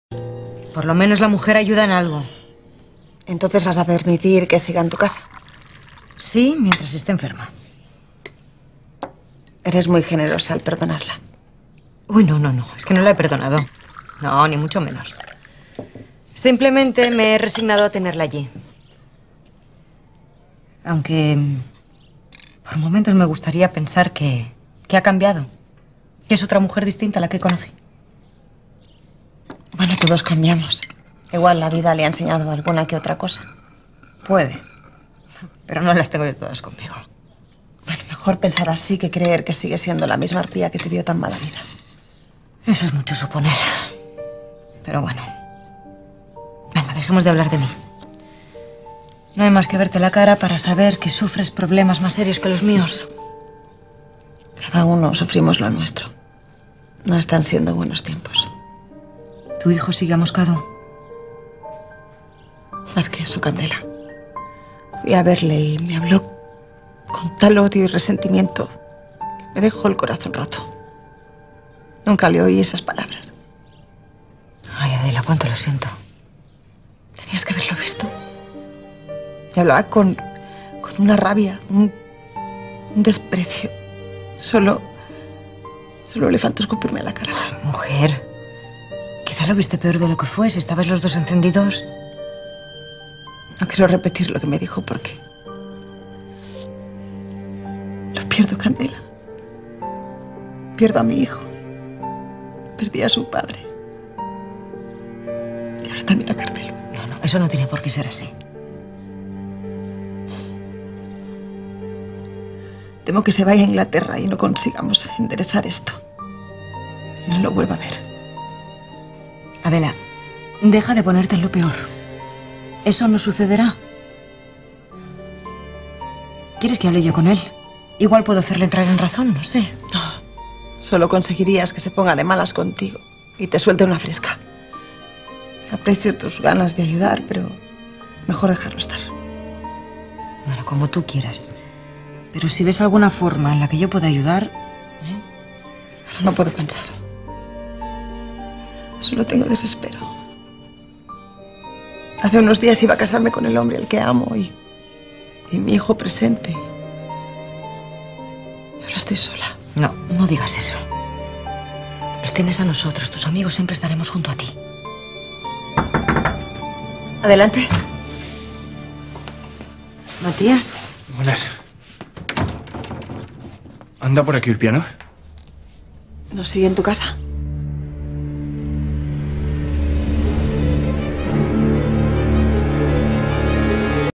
La dolorosa confesión de Adela y su corazón roto
Como dos buenas amigas, Adela y Candela han compartido un te con el que contarse sus problemas y todo aquello que les atormenta. Adela dice tener el corazón roto al sentir que pierde al único hombre de su vida que le quedaba: su hijo. Candela no puede sino darle palabras de aliento a su amiga y asegurarle que todo va a salir bien.